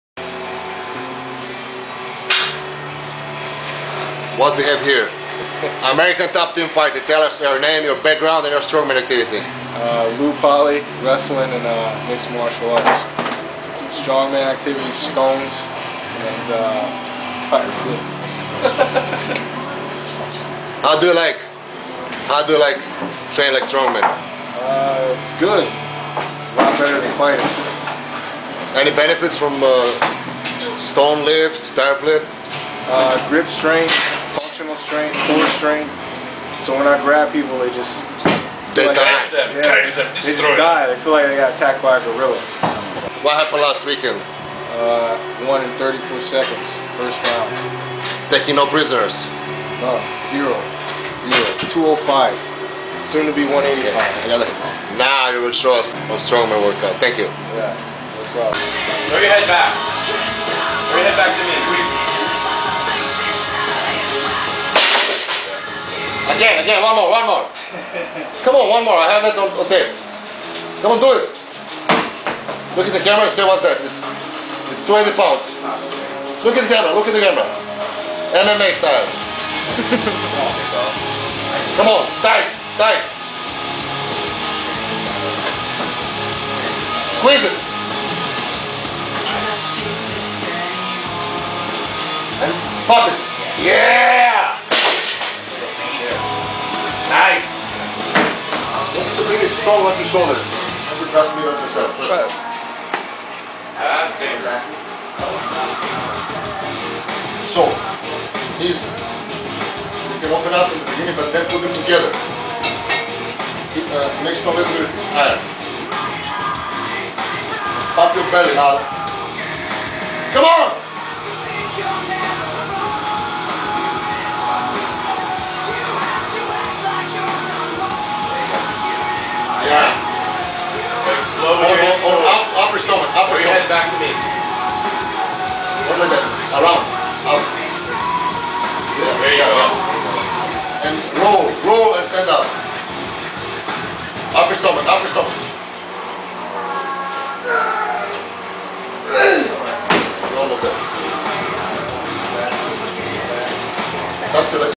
Lew Polly the strongman in MMA sprt
Awesome guy, MMA fighter train with us strongman style. Lifting stones up to 275 lbs, farmer walk with 250 lbs in each arm

Stay posted he has great future!